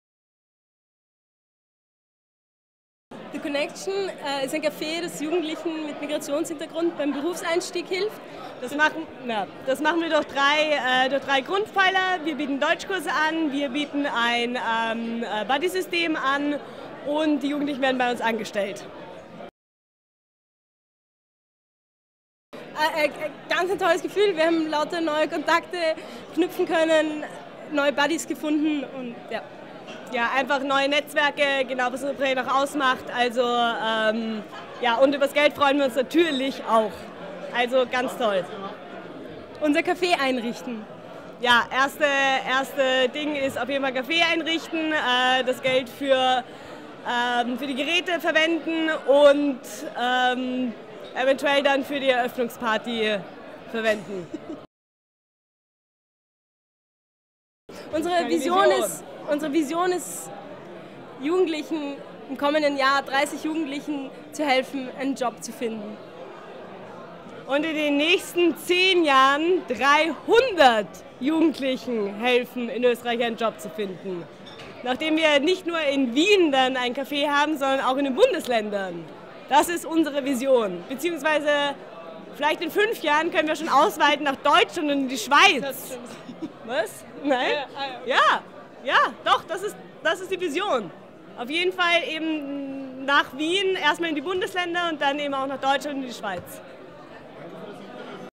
Social Impact Award Winning Project 2011: The Connection
The Connection ist ein Projekt, das mit Hilfe eines Cafes Menschen mit Migrationshintergrund bei der Integration hilft. Dies geschieht zum einen über Deutschkurse, zum Anderen über ein Angebot von Arbeitsplätzen.